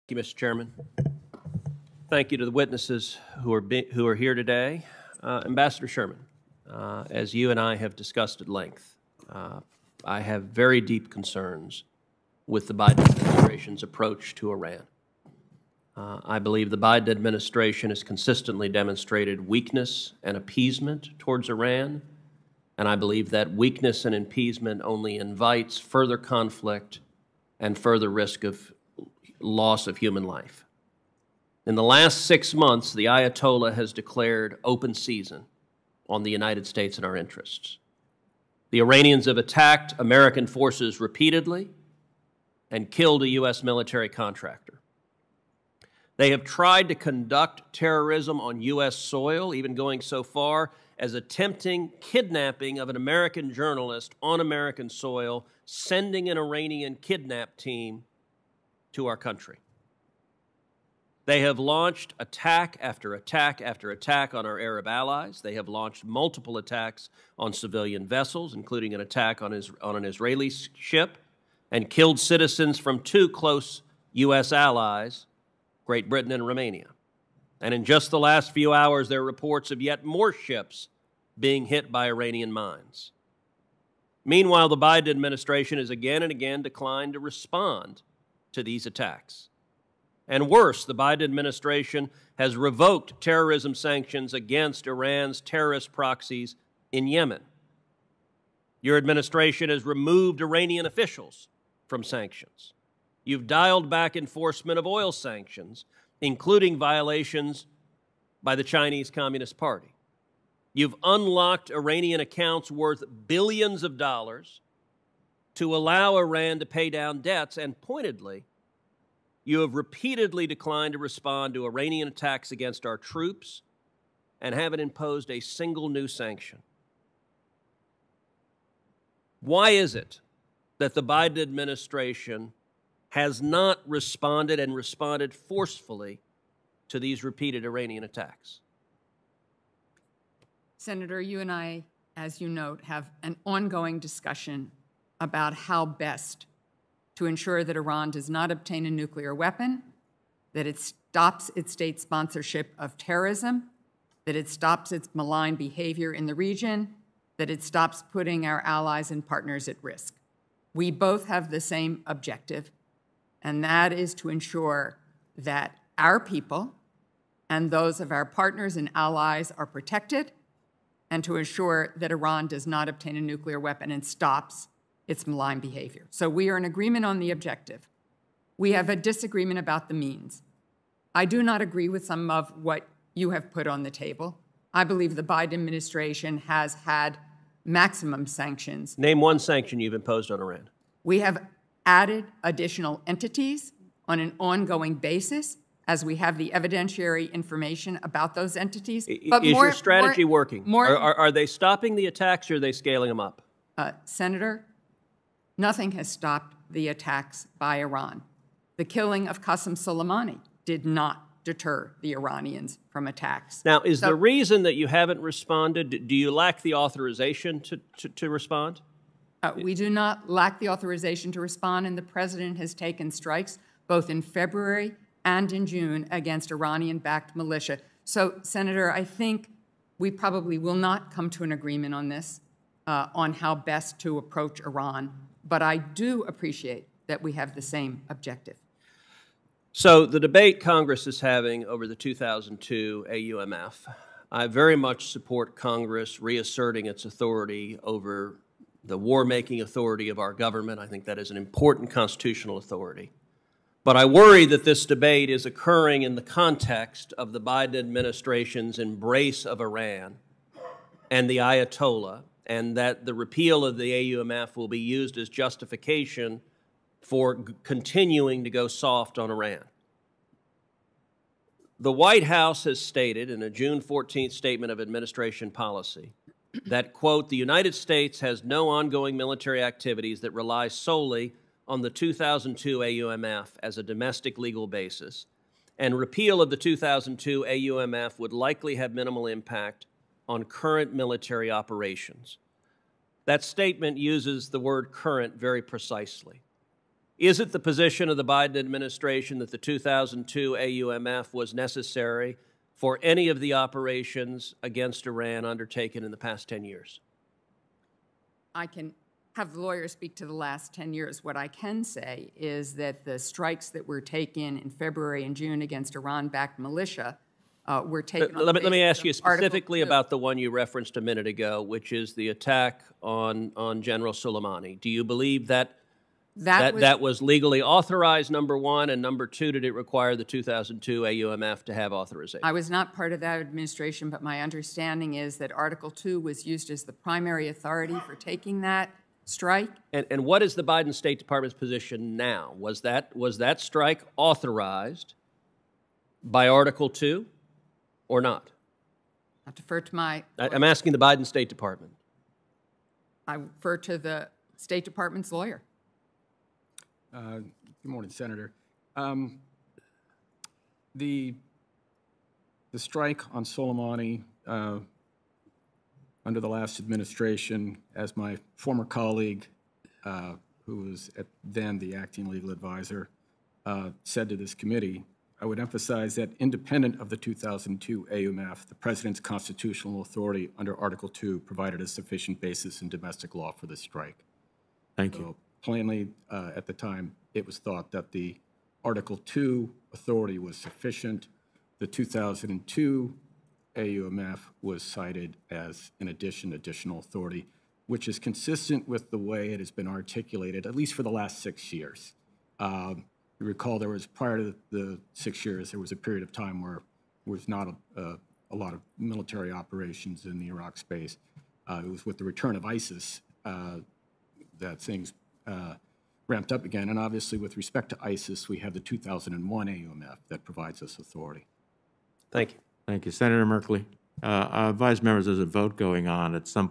'Name One Sanction You Have Put On Iran': Ted Cruz Clashes With Biden Official During Hearing
During today's Senate Foreign Relations Committee hearing, Sen. Ted Cruz (R-TX) castigated Deputy Secretary of State Wendy Sherman for the Biden Administration's posture toward Iran.